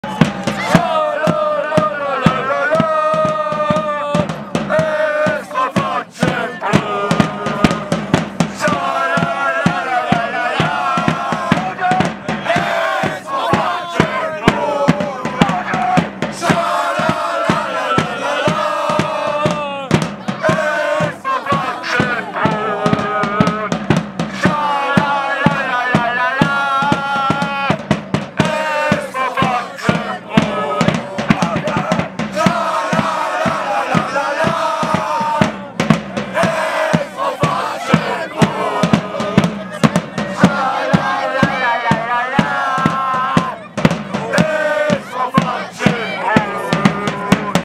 Video Fatschenbrunn
Die Fans der "Fatschis", des SV Fatschenbrunn, beim Relegations-Fußballspiel in Prappach gegen den SV Hofheim